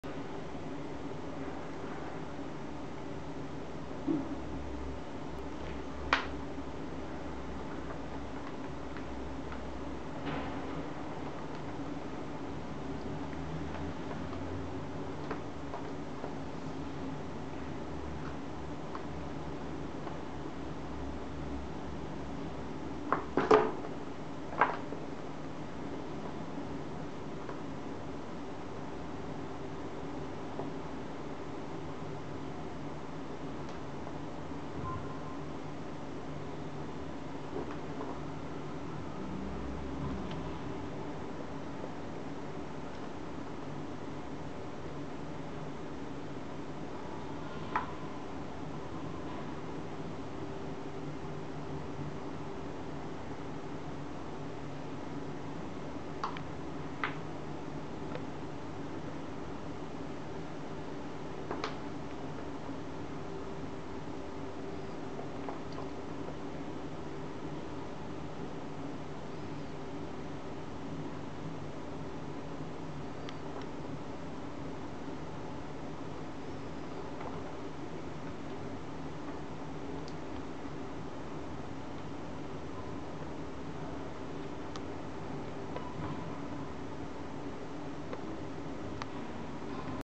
Pet Rats: Sheephead trying to jump up the wall!
Free range time again today and I added something new for them to explore; Their bath tub and a DIY coroplast ladder (There was no water in the tub though).